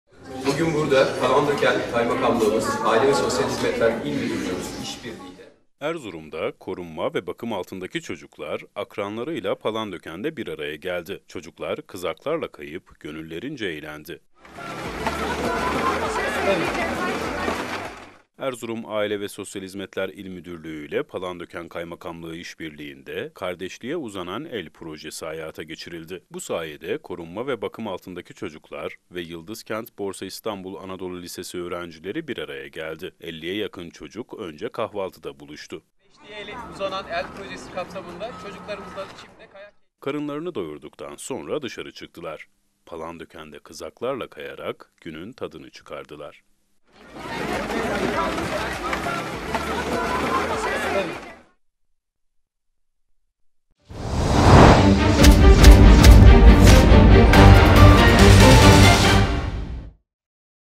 Kardelen TV | KARDEŞLİĞE UZANAN EL PROJESİ'NDE BİR ARAYA GELDİLER
Erzurum’da Korunma Ve Bakım Altındaki Çocuklar, Akranlarıyla Palandöken’de Bir Araya Geldi. Çocuklar, Kızaklarla Kayıp Gönüllerince Eğlendiler…